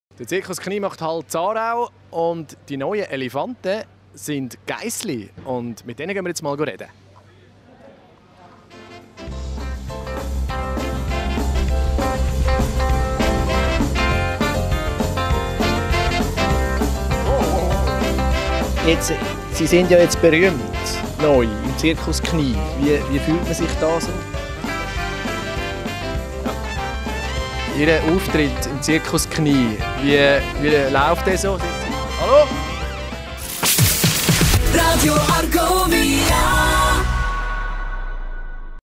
Marius Füglister besucht den Circus Knie
Der Circus Knie kommt nach Aarau. Die perfekte Gelegenheit für Marius, die richtigen Stars des Abends kennenzulernen. Franco, Chris und ihre Geissli heizen dem Publikum nämlich mit einer tollen Show ein. Ob die Ziegen Marius ihre Geheimnisse wohl verraten?